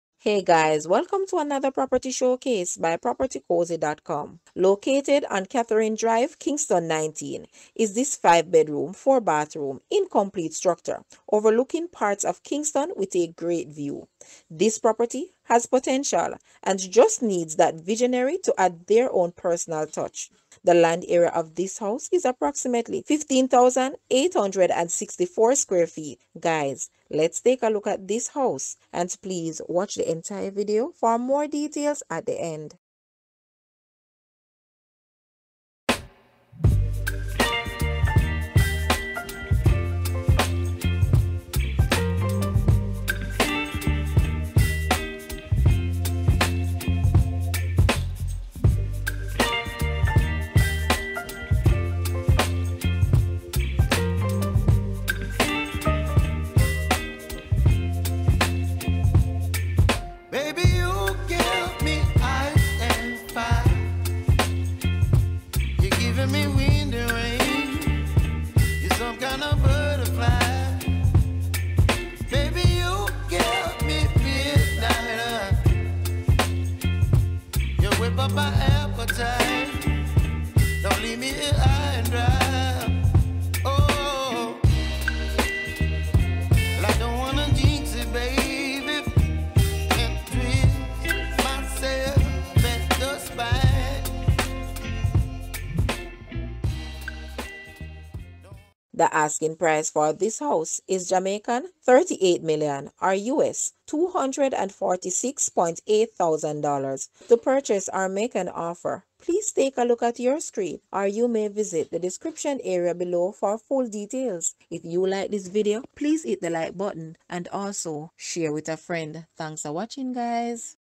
Unfinished 5 Bedroom 4 Bathroom House for sale on Catherine Drive, Kingston & St Andrew, Jamaica
5 bedroom 4 bathroom incomplete structure overlooking parts of Kingston with a great view. This property has potential and just needs that visionary to add their own personal touch.

PRICED AT JMD$38,000,000 OR USD$246,898

AGENT DETAIL
Shanique Sinclair
ID: MLS-67042